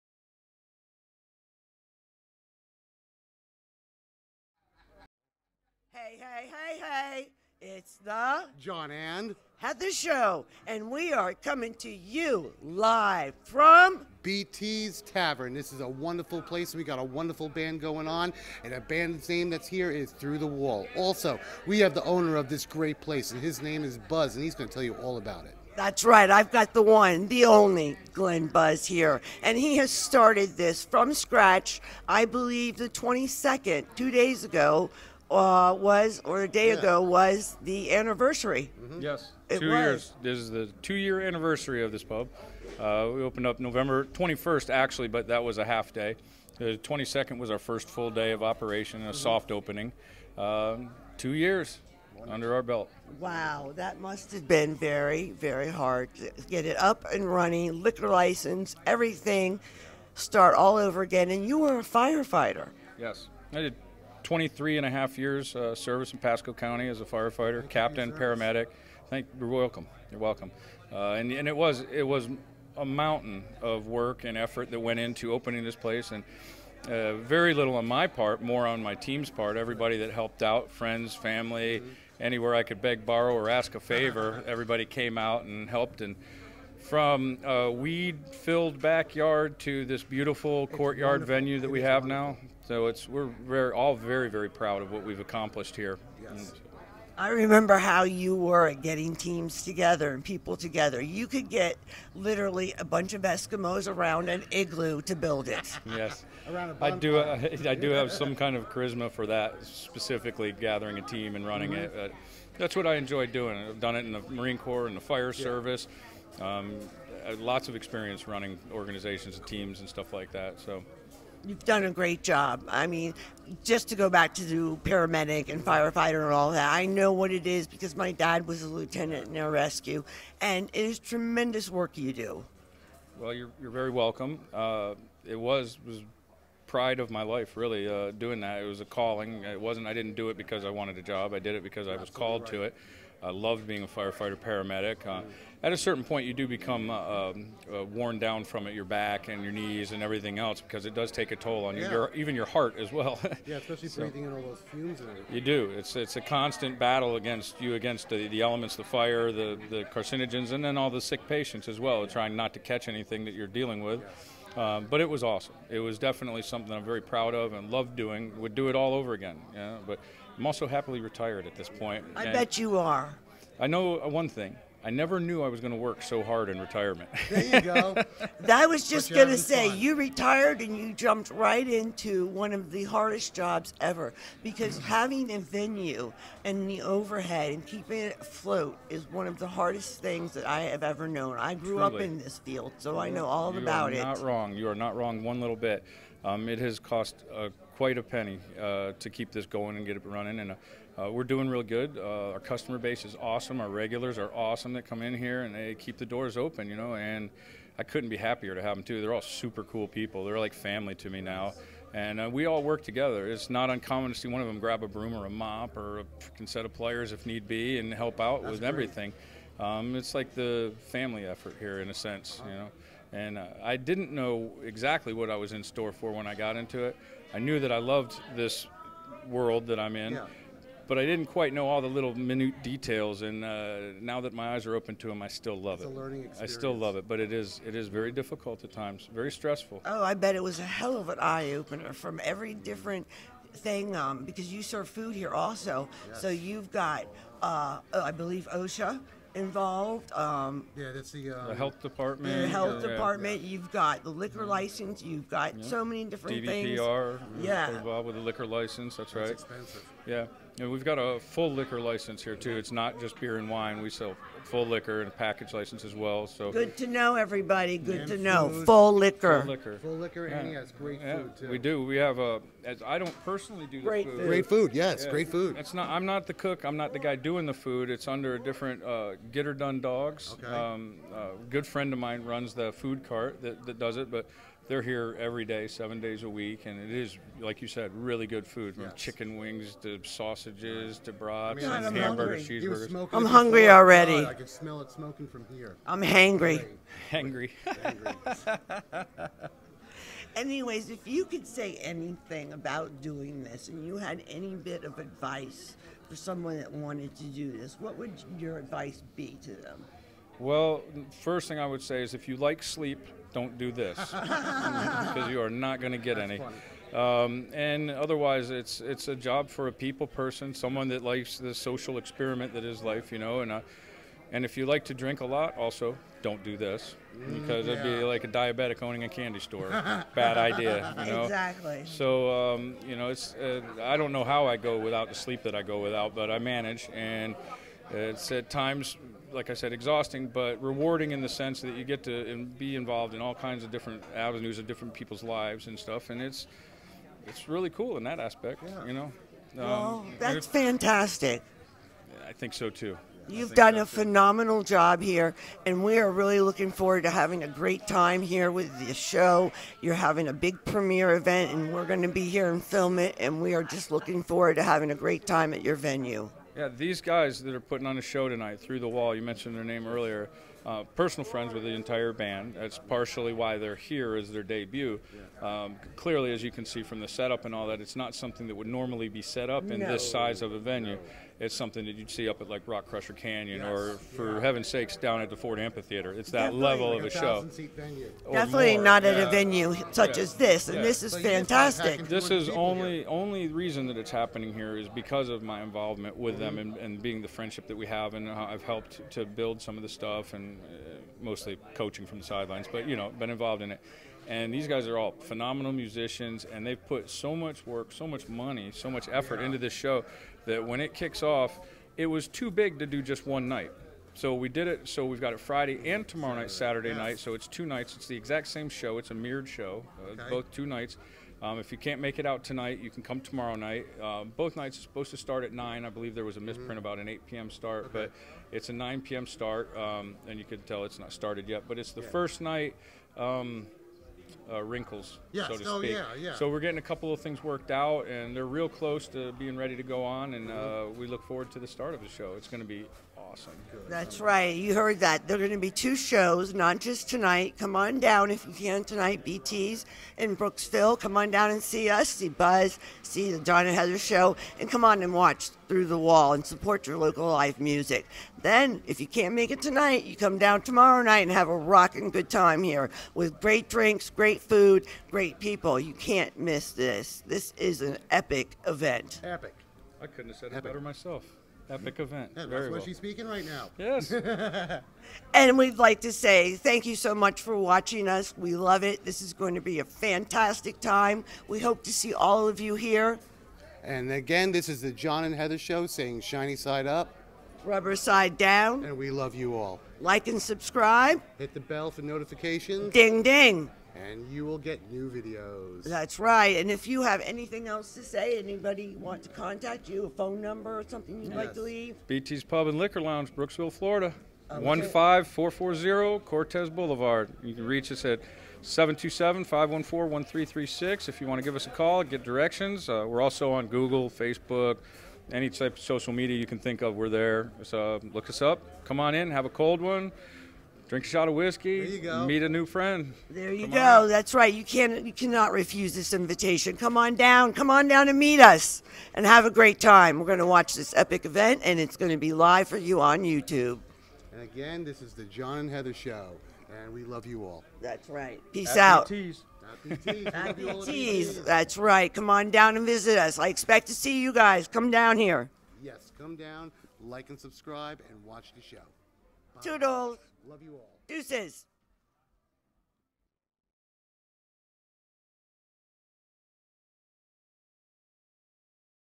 B T 's Pub Interview with Glen Buzze
The John & Heather Show go to B.T. 's Pub & Liquor lounge for a good time out on date night. We find a great venue with pool tables, karaoke, T.V.s, full liquor bars. great fresh food, outside bar, fire pit, horshoes, cornhole and a very large stage for bands.
Over 2,000 Videos of Bands

BT's Pub and Liquor Lounge